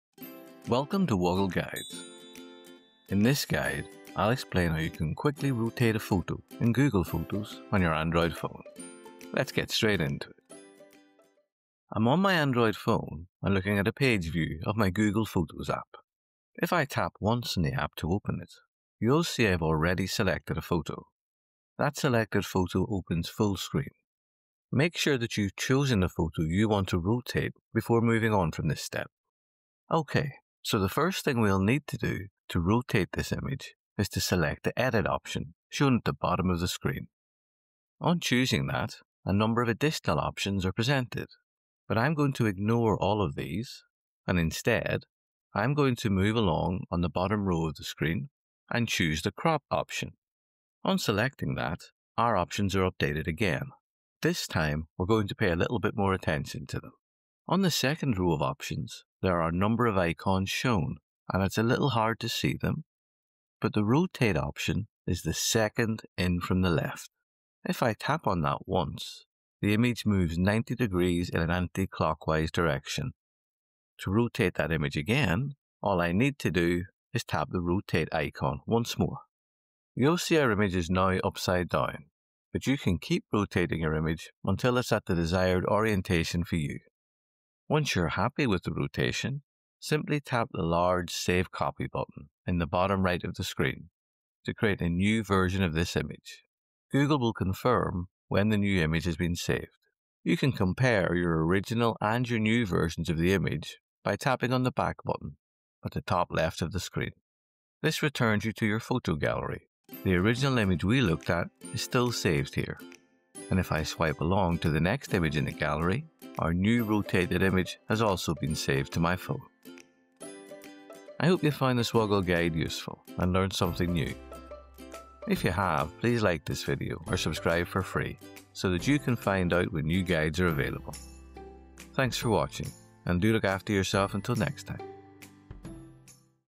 How to rotate a photo in Google Photos on Android
In this guide, I’ll explain how you can quickly rotate a photo in Google Photos on your Android phone.

🕔 Key Moments

00:00 | Introduction
00:19 | How to rotate a photo in Google Photos on Android
01:36 | Wrap up

🎵 Attribution